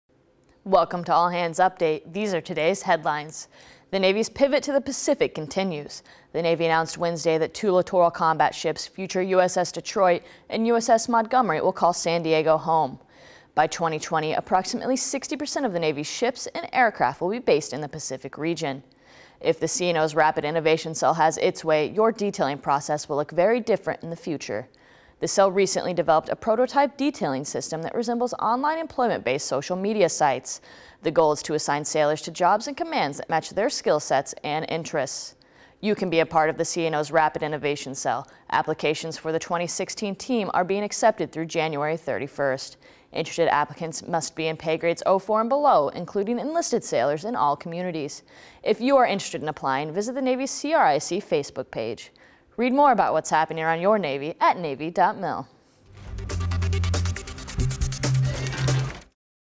Headlines for Monday, December 28, 2015 (HL-28)
San Diego Gets 2 LCS, LinkedIn Detailing, CNO Rapid Innovation Cell Applications